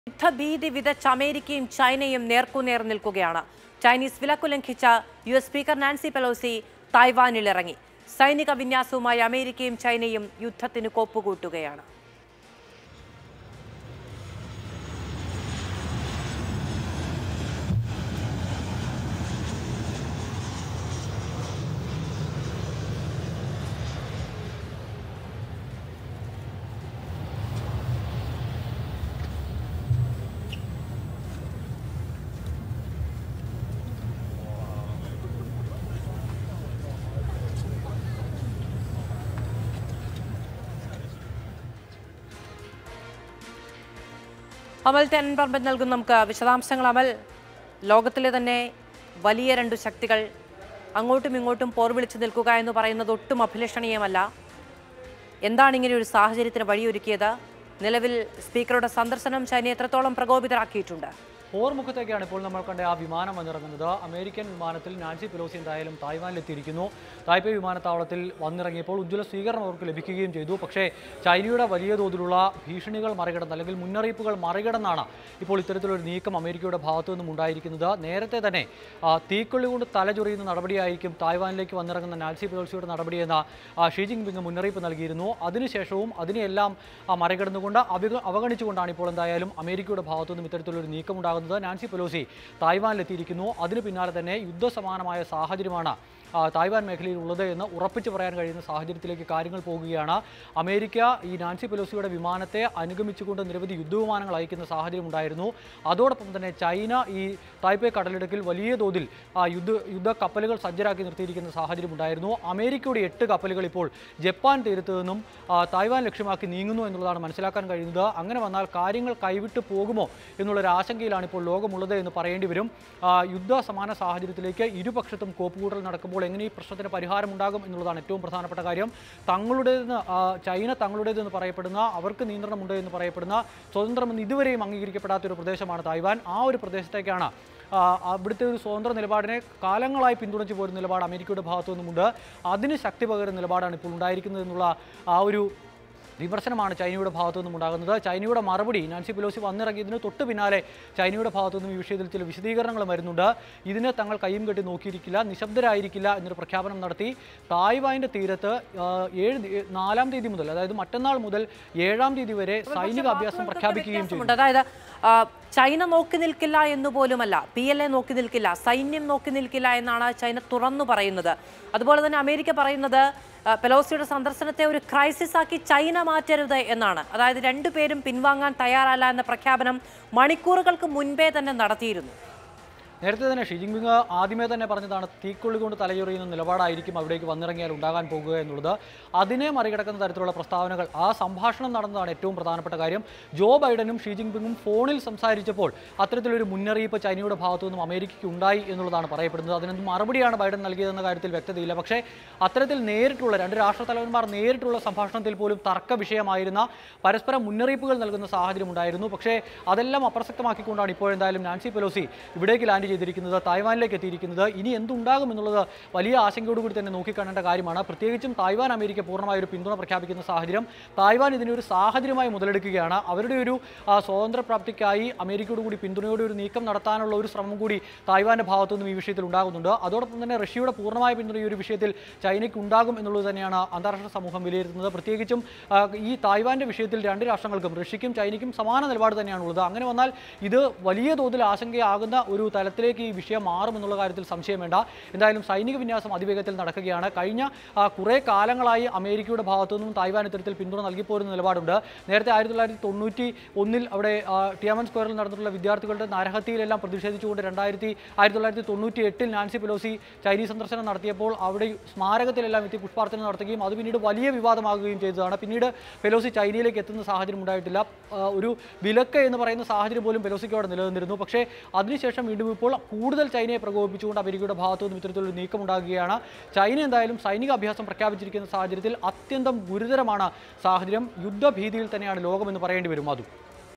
ലോകം യുദ്ധഭീതിയിൽ: അമേരിക്കയും ചൈനയും നേർക്കുനേർ | Mathrubhumi News
ചൈനീസ് വിലക്ക് ലംഘിച്ച് യുഎസ് സ്പീക്കർ നാൻസി പെലോസി തായ്വാനിലിറങ്ങി. സൈനിക വിന്യാസവുമായി അമേരിക്കയും ചൈനയും യുദ്ധത്തിന് കോപ്പുകൂട്ടുന്നു.

Happy viewing!